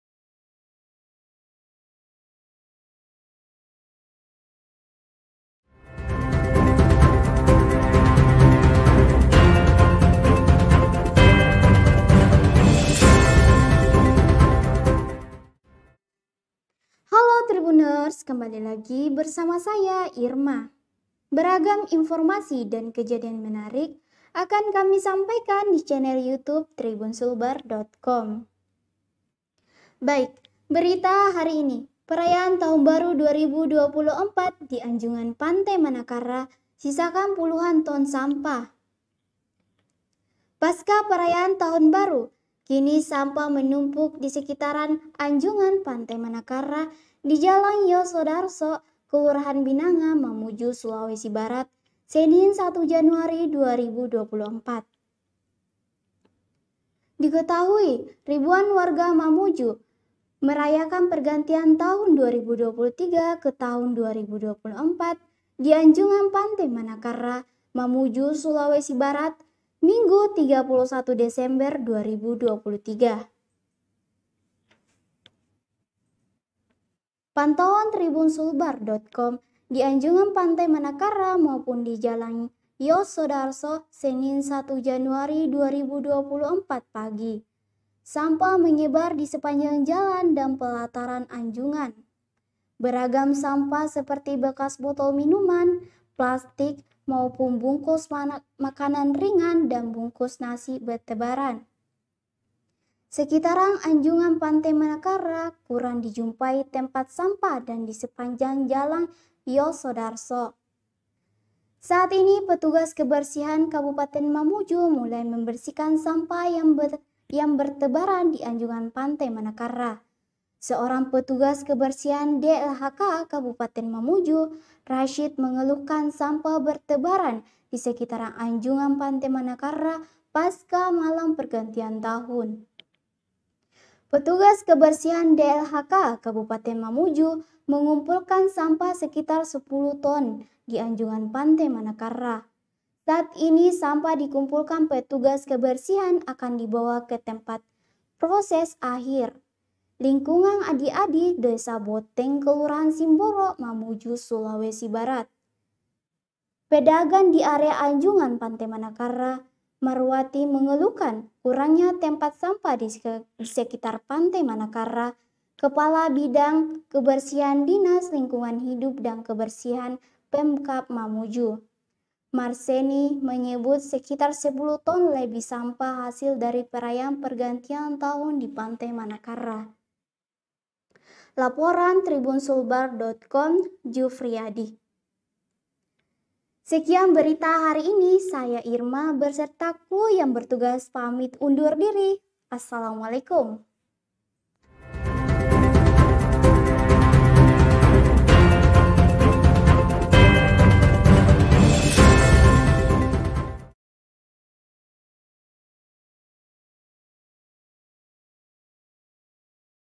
Perayaan Tahun Baru 2024 di Anjungan Pantai Manakarra Sisakan Puluhan Ton Sampah
TRIBUN-SULBAR.COM, MAMUJU - Pasca pesta perayaan tahun baru, kini sampah menumpuk di sekitar Anjungan Pantai Manakarra, Jl Yos Sudarso, Kelurahan Binanga, Mamuju, Sulawesi Barat (Sulbar), Senin (1/1/2024).

Diketahui, ribuan warga Mamuju, merayakan pergantian tahun 2023 ke tahun 2024 di Anjungan Pantai Manakarra, Mamuju, Sulawesi Barat, Minggu (31/12/2023).

Artikel ini telah tayang di Tribunsulbar.com dengan judul Perayaan Tahun Baru 2024 di Anjungan Pantai Manakarra Sisakan Puluhan Ton Sampah,
Penulis: Jufriadi | Editor: Nurhadi Hasbi

HOST: IRMA
OPERATOR: NURSAKIA
GRAFIS: AFIFAH WAHID